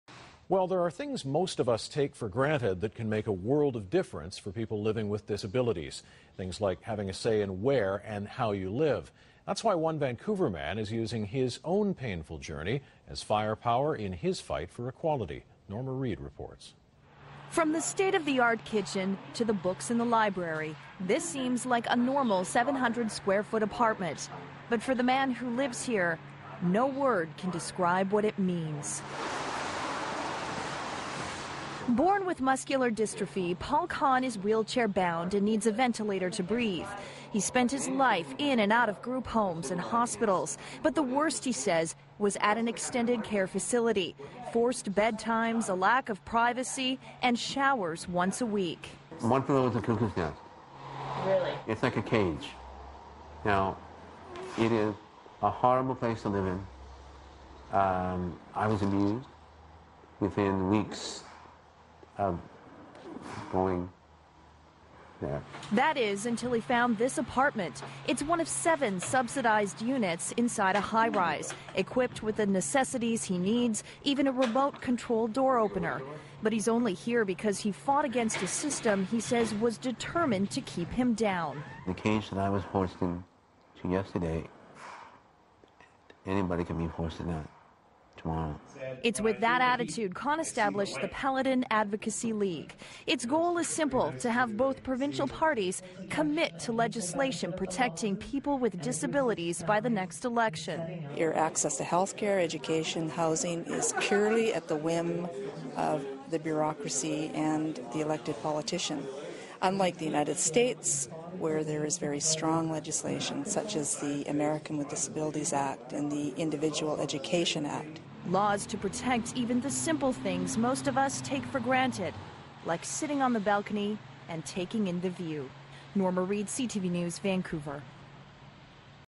People with Disabilities and Institutional Violence - Paul's Story
This report out of Vancouver (but it could be most anywhere in Canada) demonstrates the violence of institutional bureaucracies against people with disabilities. The humane and respectful solution to housing for people with disabilities is not that complicated. We just need to do it! And stop putting people in institut...ions. We will all need some help one day - are you ready to live on one shower a week? And that's not the worst of it - just one obvious example of how your quality of life could be pulled out from under you overnight if you were condemned to such a life.